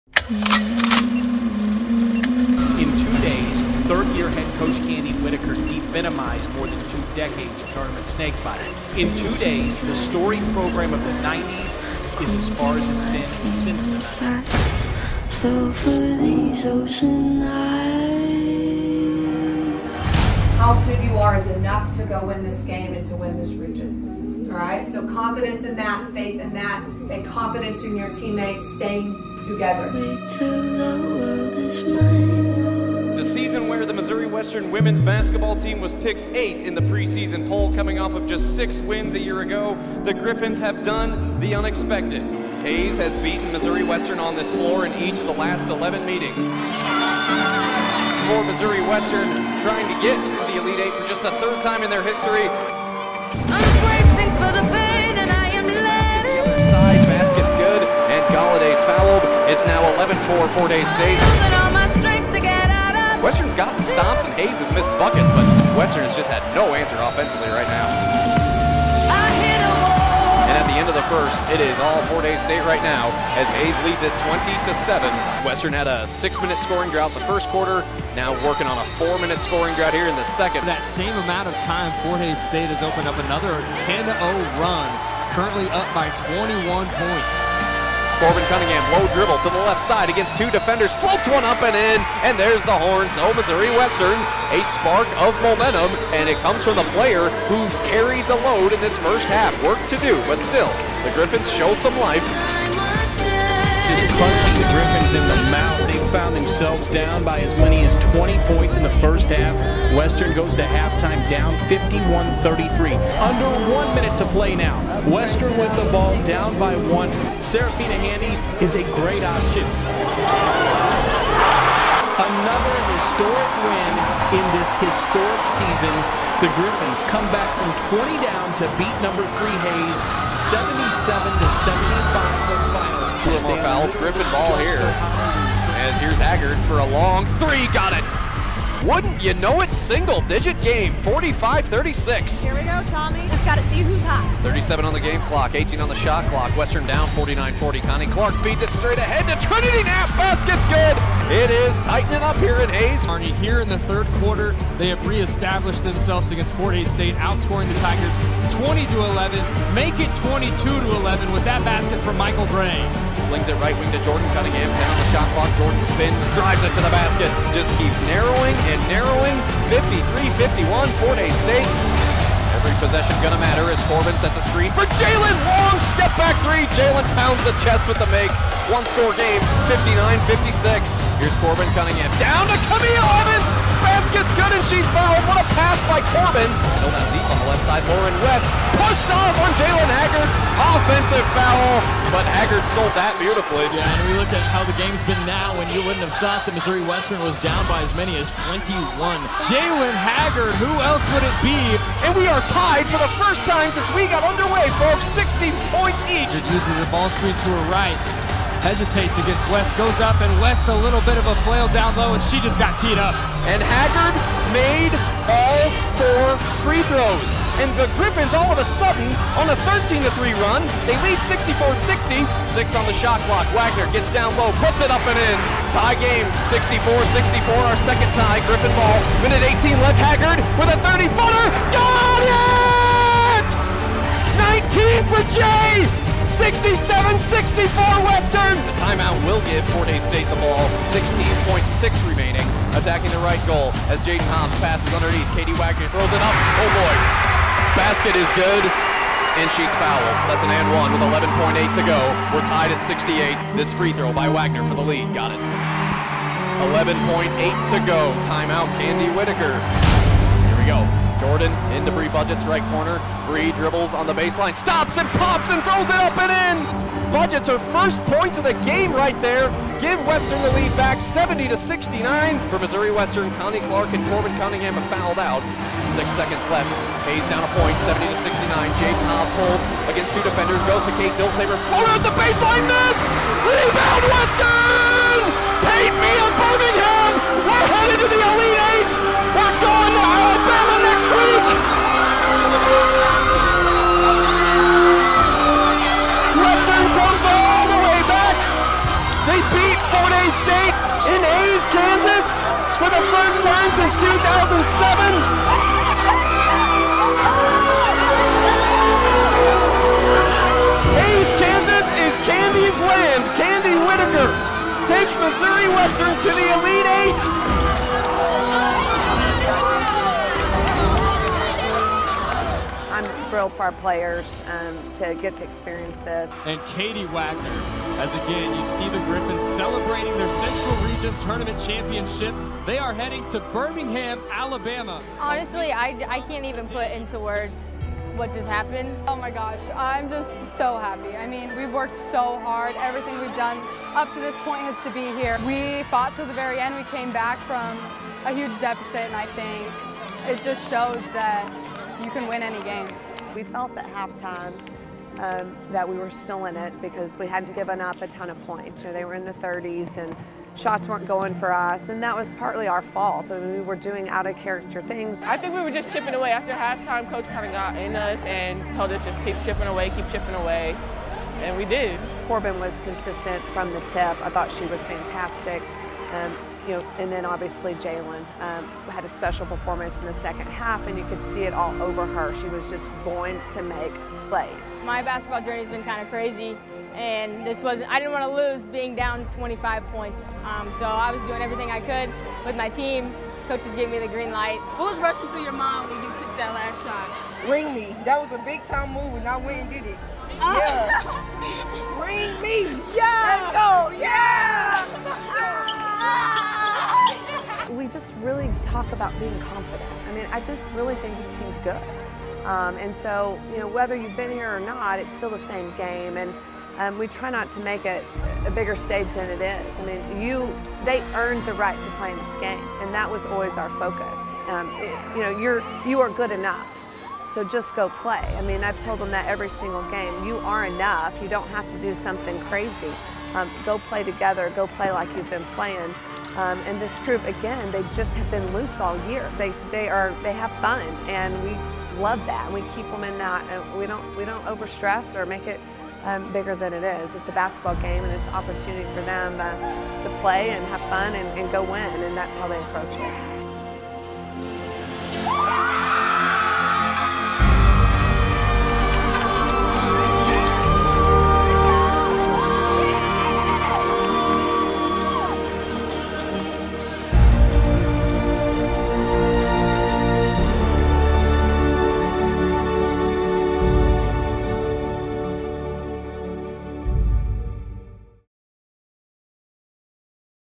3-14-22: The Greatest Comeback in Missouri Western history.
Down 21 in the 2nd quarter of a Sweet 16 game on Hays' home floor, the Griffon Women completed another chapter in this unthinkable storybook season.  The story is still being written.